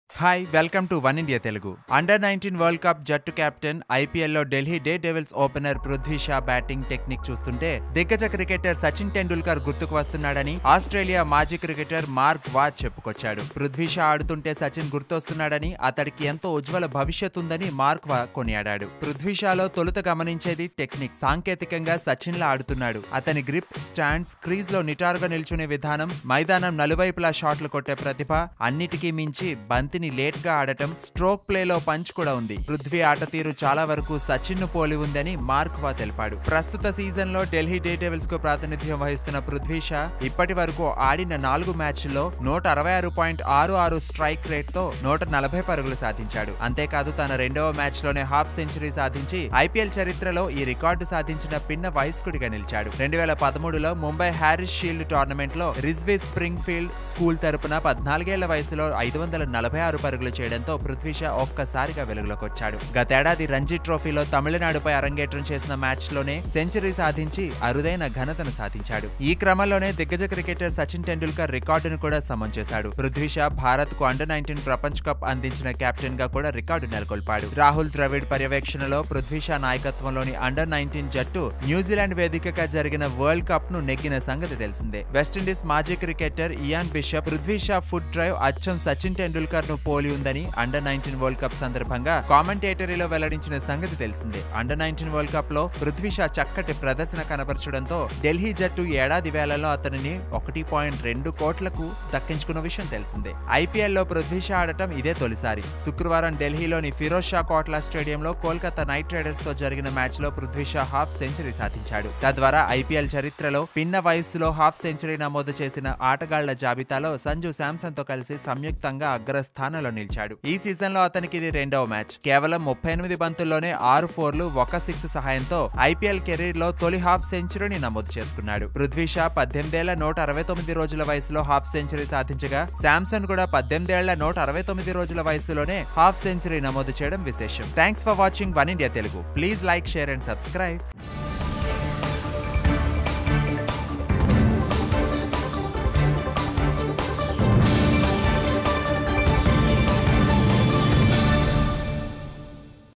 IPL 2018 : Prithvi Shaw's Technique is Like Sachin | Oneindia Telugu
Former Australian cricketer Mark Waugh believes Delhi Daredevils opener Prithvi Shaw's batting technique resembles that of cricket icon Sachin Tendulkar.
అండర్-19 వరల్డ్ కప్ జట్టు కెప్టెన్, ఐపీఎల్‌లో ఢిల్లీ డేర్‌డెవిల్స్ ఓపెనర్ పృథ్వీ షా బ్యాటింగ్‌ టెక్నిక్‌ చూస్తుంటే దిగ్గజ క్రికెటర్ సచిన్‌ టెండూల్కర్ గుర్తుకు వస్తున్నాడని ఆస్ట్రేలియా మాజీ క్రికెటర్ మార్క్‌ వా చెప్పుకొచ్చాడు. పృథ్వీ షా ఆడుతుంటే సచిన్ గుర్తొస్తున్నాడని, అతడికి ఎంతో ఉజ్వల భవిష్యత్ ఉందని మార్క్ వా తెలిపాడు.